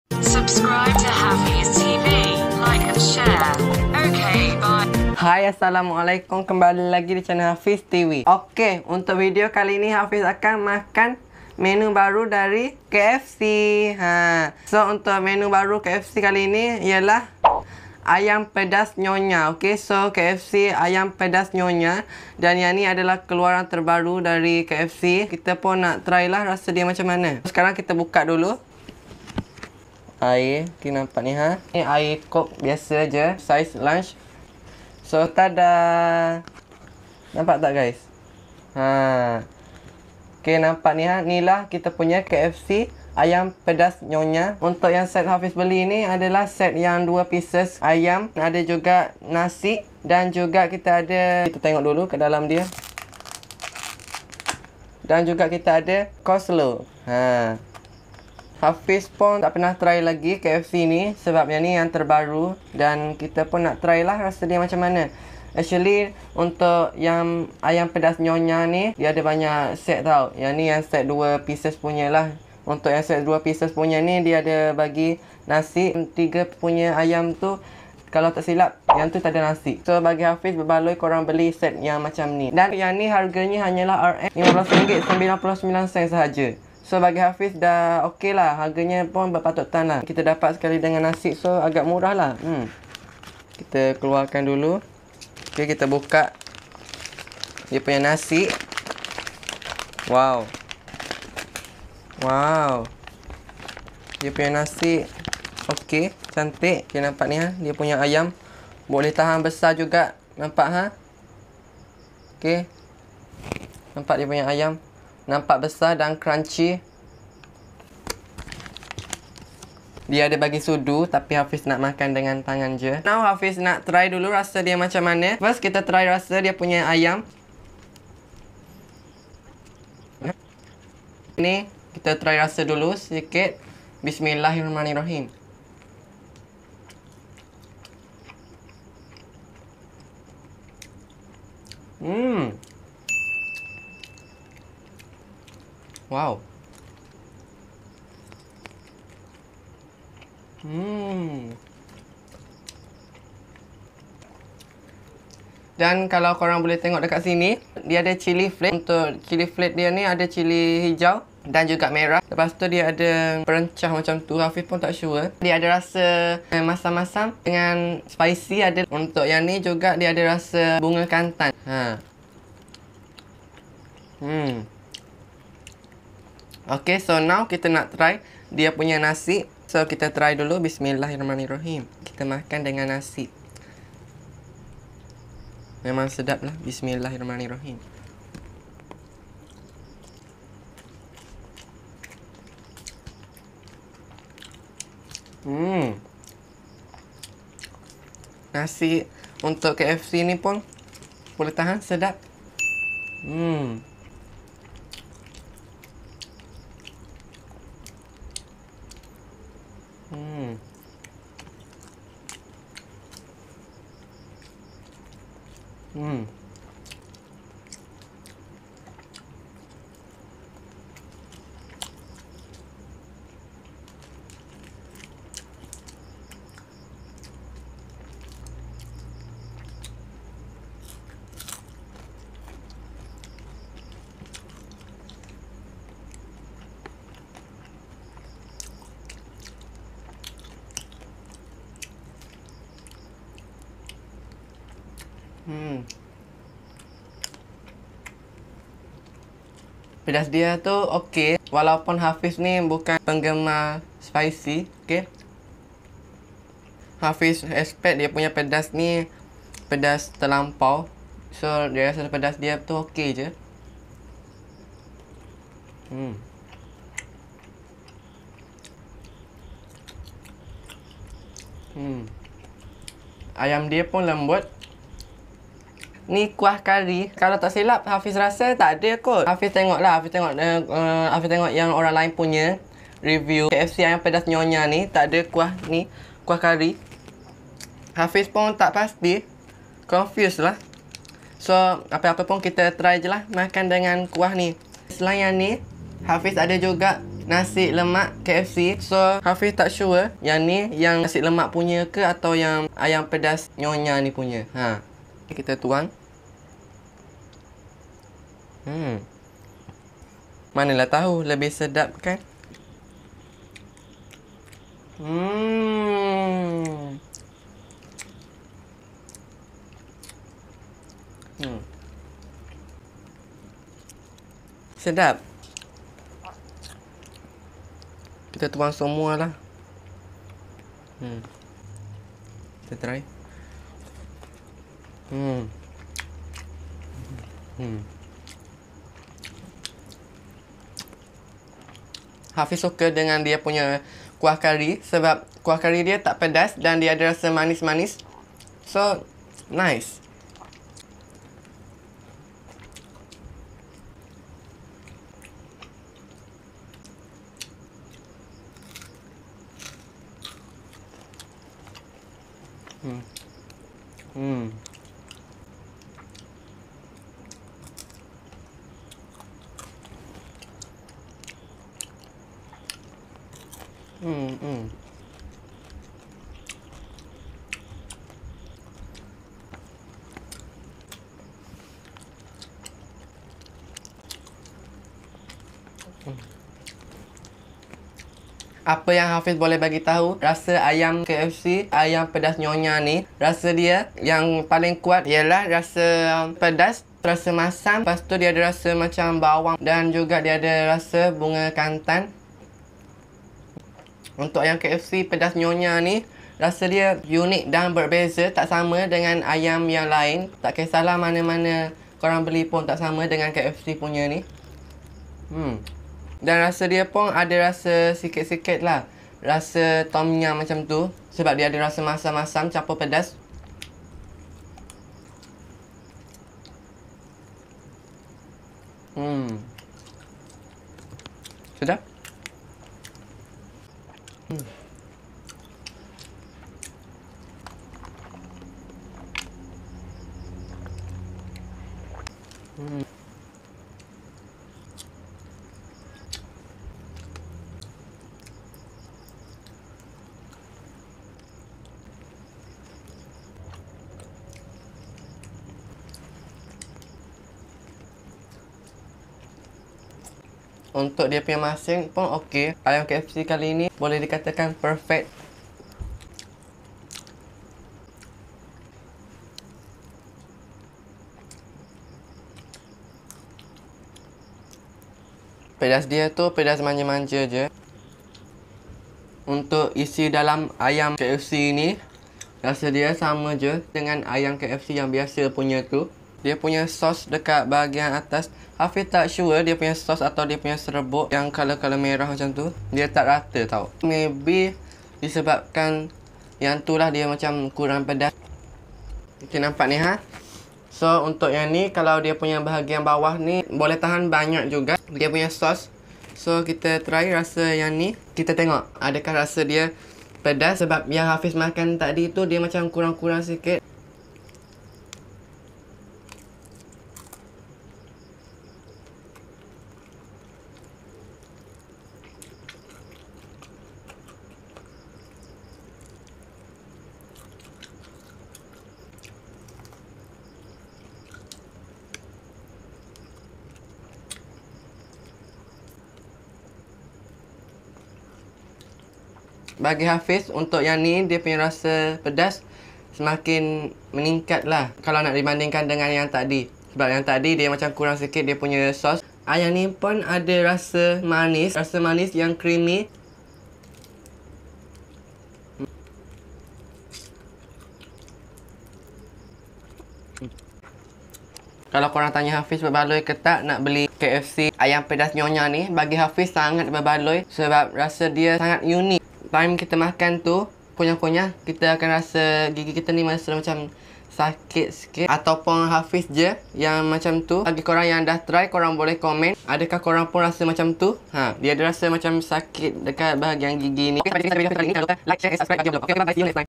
NEW! KFC AYAM PEDAS NYONYA | PEDAS SANGAT KE?!
Hai Guys! Video kali ni hafiz akan makan menu terbaru dari kfc. Kfc keluarkan menu kfc ayam pedas nyonya! So, memang pedas ke? Nak tahu lebih detail tengok video ni sampai habis.
*Harga : RM15.99 (Harga walk-in sahaja)
2 pcs ayam, colonel rice, coleslaw, drink
Jangan Skip Iklan Tau! Thank You So Much Geng!!